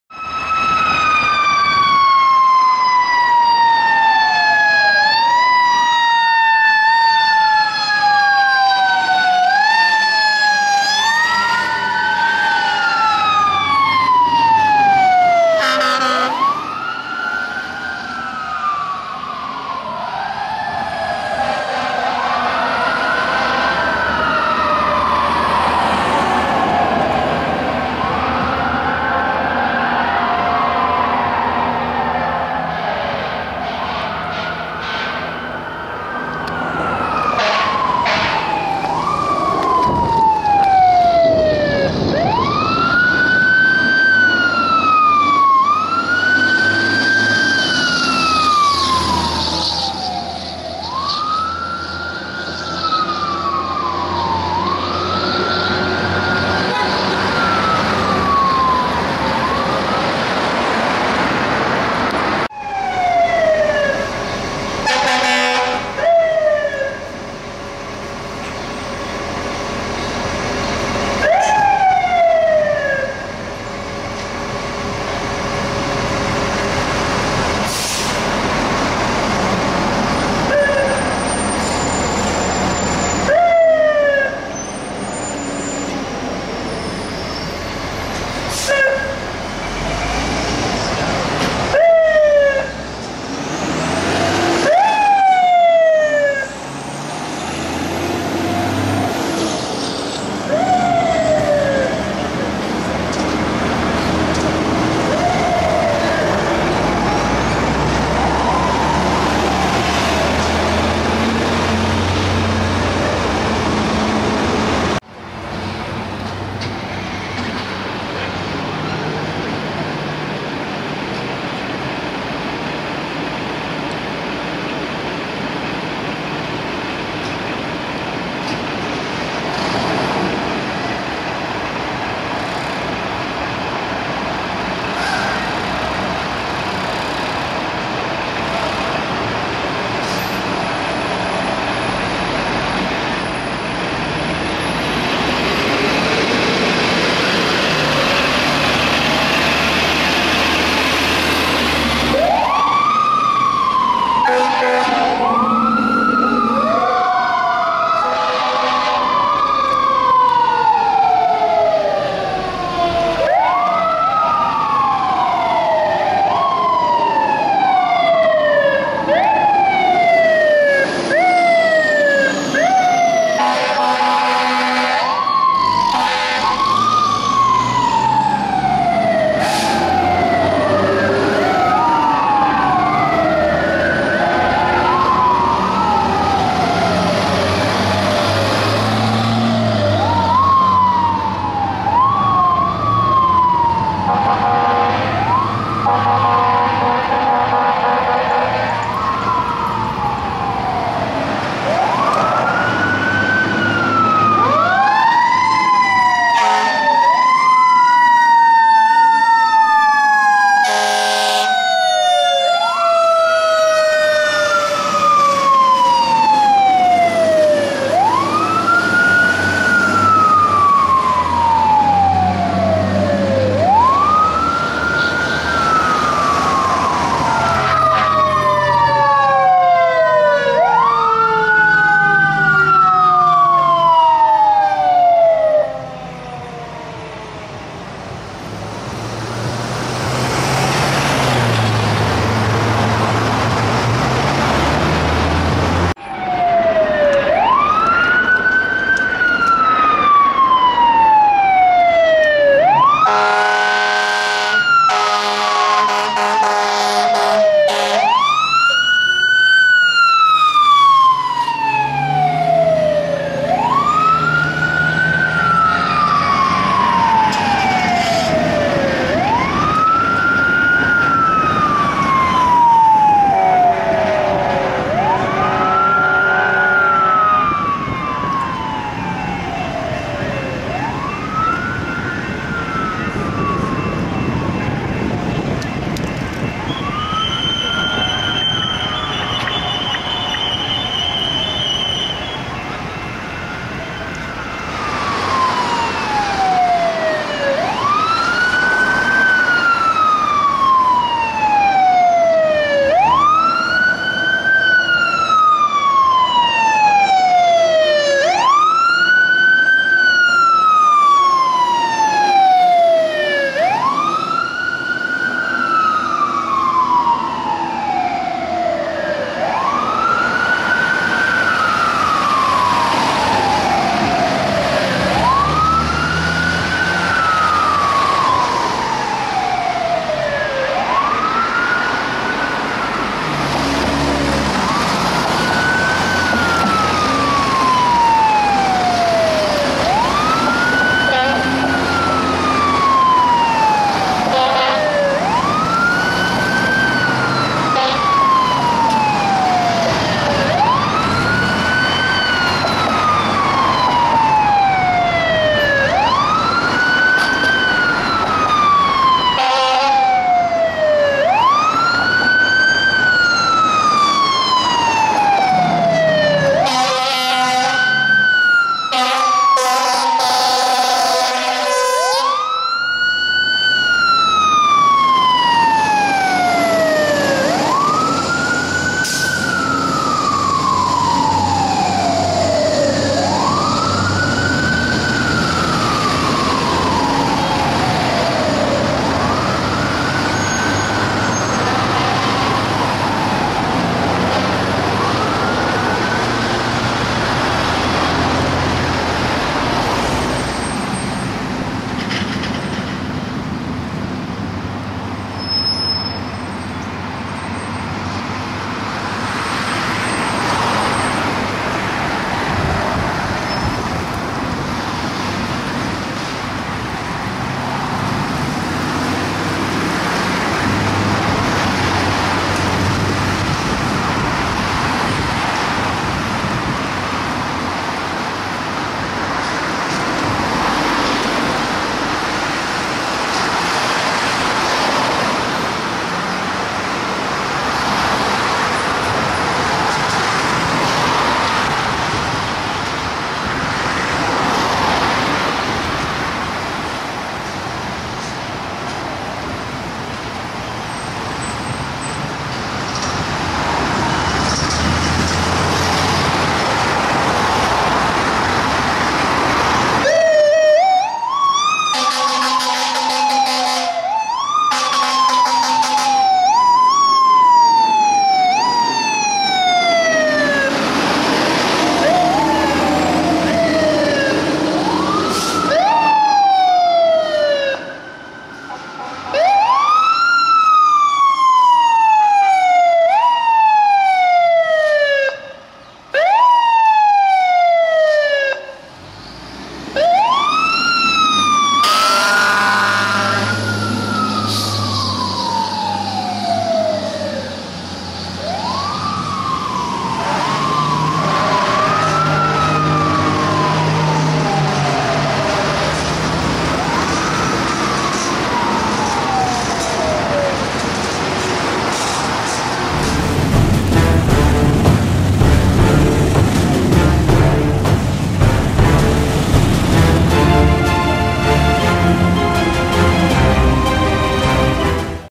Fire Trucks & Engines Responding Compilation: Sirens, Air Horns - LAFD
A best of compilation of fire trucks and engines responding in the city of Los Angeles, California. In this video you'll see "Light Force" 29, 27 and 61 including the rare capture of yellow Engine 51 at Los Angeles International Airport (LAX) responding lights, sirens and air horns. There's also Engine 4, Engine 52 and tiller ladder 27 which runs out of compressed air for the air horns at 06:11. Lots of air horn usage!